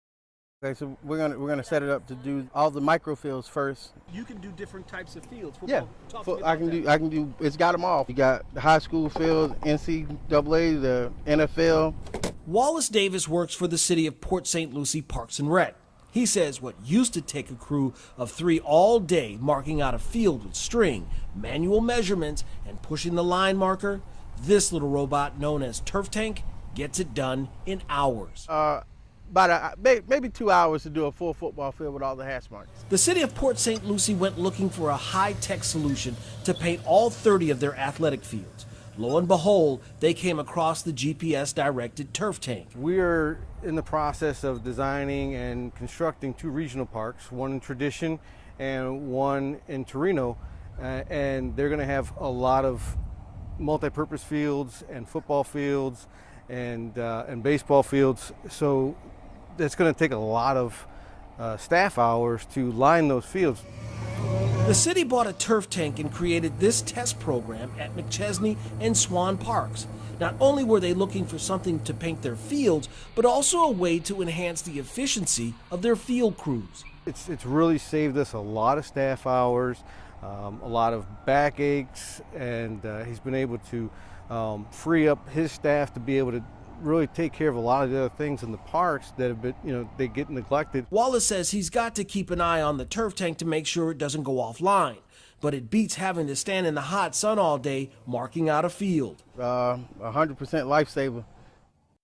Turf Tank robot makes job of painting football fields easier
There's a robotic device that takes much of the sweat work out of marking the lines of a football field. Port St. Lucie is already employing the Turf Tank at two of its city parks.